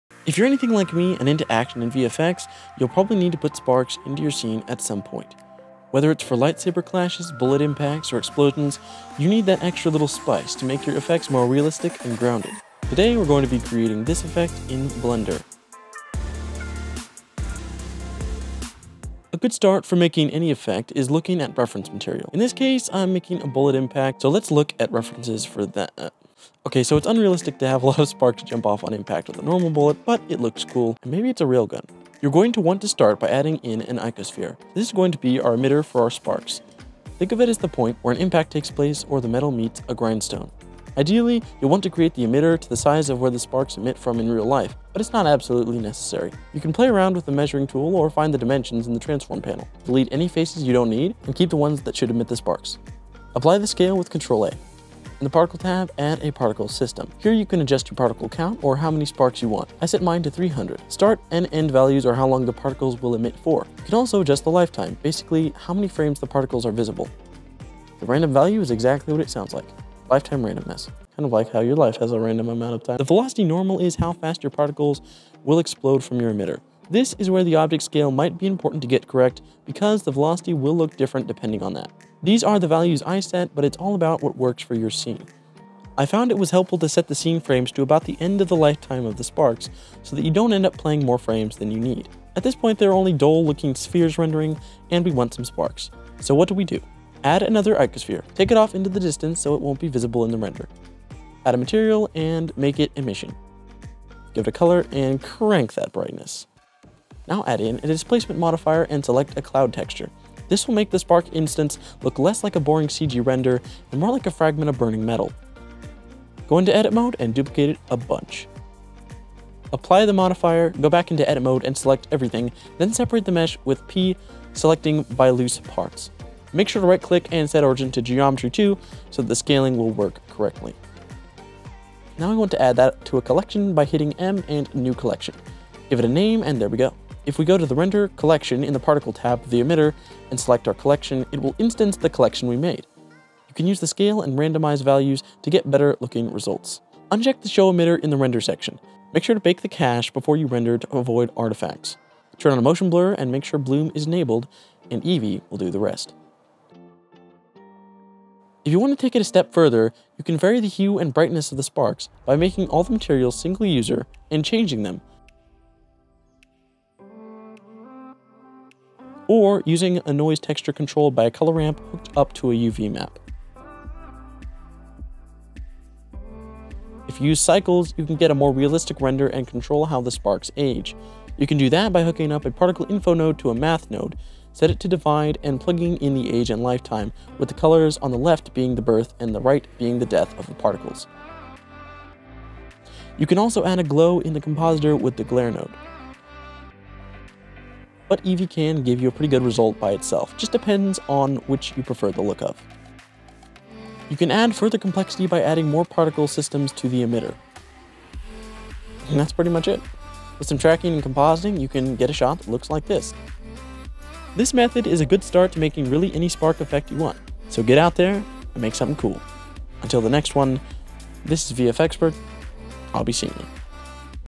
How to Make Realistic Sparks | Blender Tutorial
If you've ever wanted to make sparks because you've run out of online resources or need a specific type, then this video might be for you. Hope it helps you make your own awesome stuff!

A Light in the Dark
John 8:12